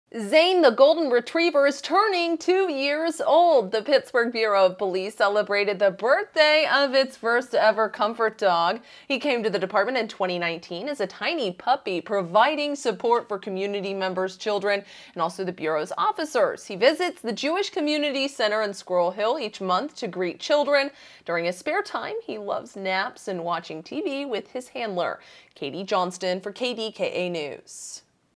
Pittsburgh Police Comfort Dog, Zane, Turns 2-Years-Old
The Pittsburgh Bureau of Police’s first-ever comfort dog is celebrating his second birthday. Katie Johnston reports.